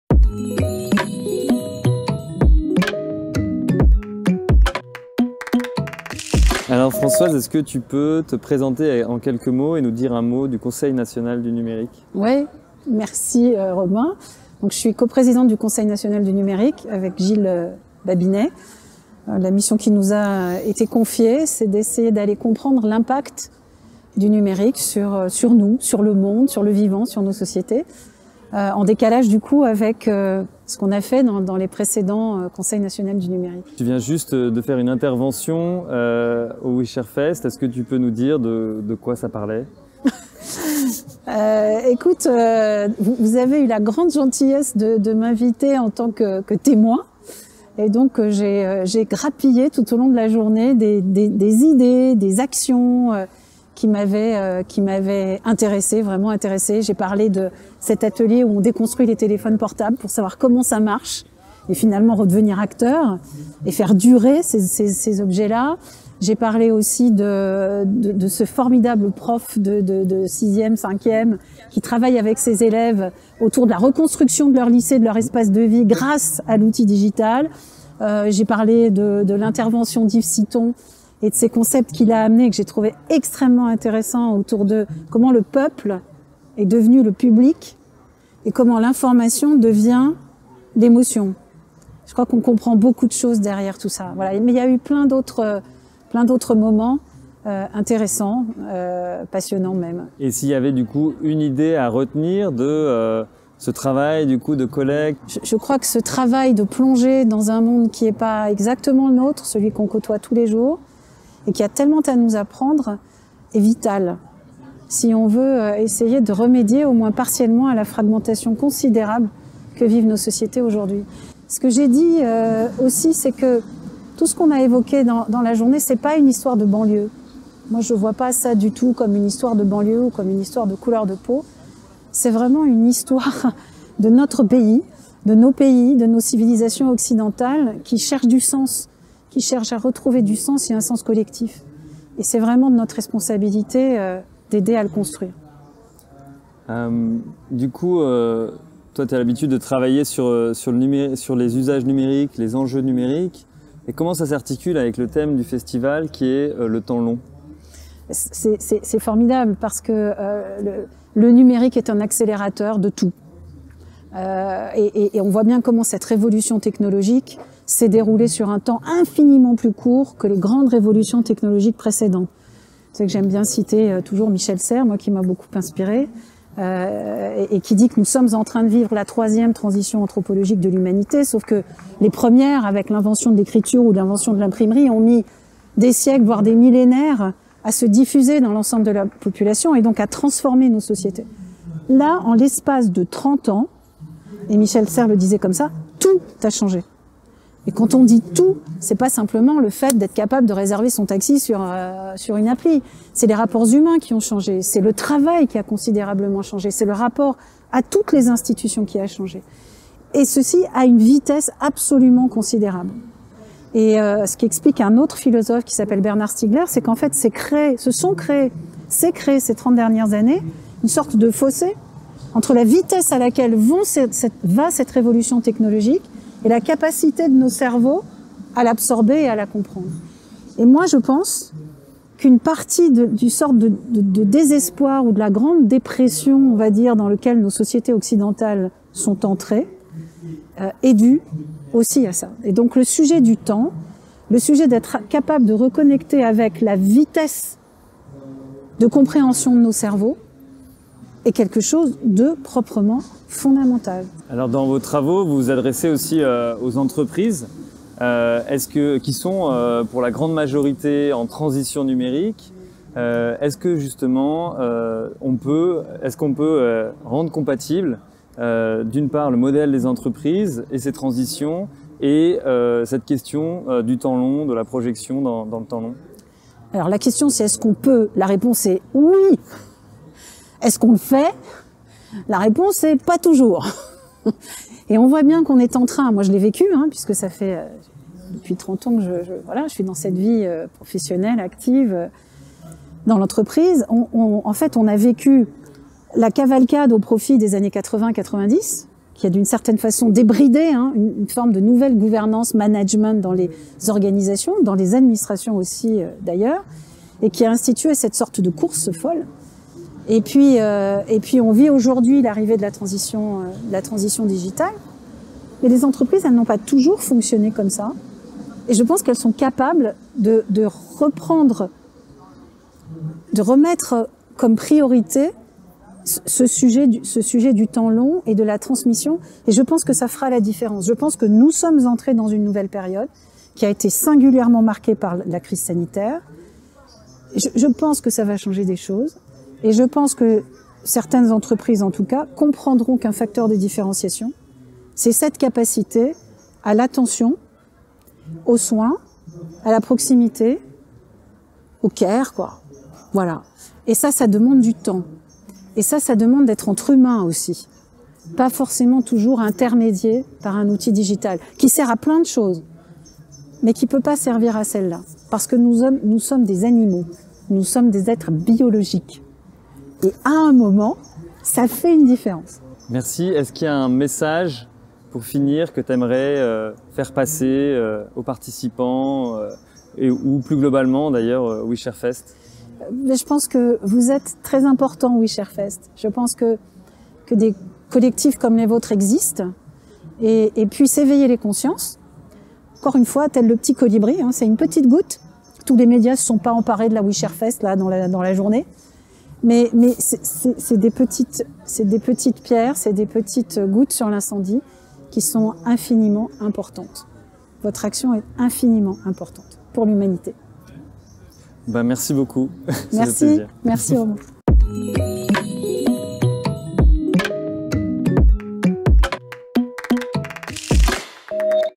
Françoise Mercadal-Delasalles - Interview Ouishare Fest 2021
Françoise Mercadal a participé au Ouishare Fest 2021 en tant que grand témoin sur les questions numériques. Dans cette interview, elle revient sur les grandes questions soulevées par le numérique aujourd’hui :
Quelle place accorder à cette technologie dans notre société ?
Comment prendre le temps de la comprendre et de l’interpréter ?